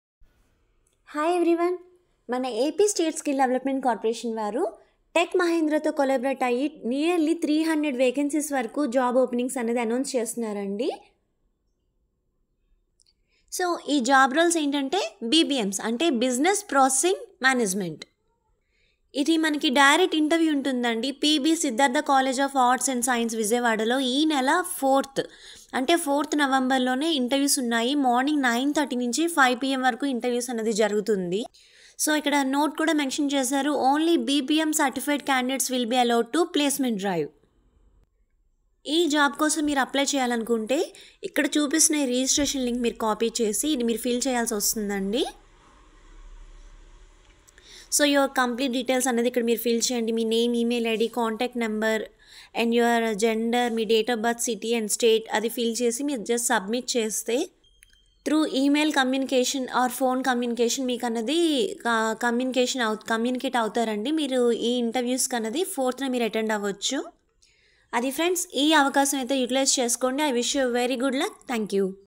Jobs in Tech Mahindra Hyderabad location
Mahindra in collaboration with APSSDC.  We are excited to offer this opportunity to your students and nearby eligible candidates, with a total of 300-400 vacancies available.

Job description:
**Job Title:** International Voice Process

**Job Type:** Immediate Joiners

**Key Responsibilities:**

* Provide customer support
* Understand and resolve customer issues and queries
* Excellent communication skills (Language proficiency B2 and above)
* Good customer handling skills

**Eligibility Criteria:**

* Education: Undergraduates, Graduates, and Postgraduates
* Work Location: Hyderabad Bahadurpally Tech Mahindra Campus
* Flexibility: 24/7 with 2 rotational offs
* 2-way cab facility provided

**Salary:** 3.5 LPA for Freshers

**Rounds of Interview:**

1. HR
2. OPS Interview

Drive Details:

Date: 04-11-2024

Venue: PB Siddhartha College of Arts & Science, Vijayawada.

Note: Only BPM Certified Candidates will be allowed to placement drive.